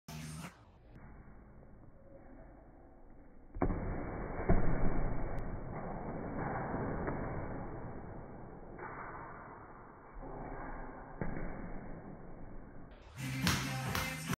The agony of defeat
The garage floor must have had a slippery spot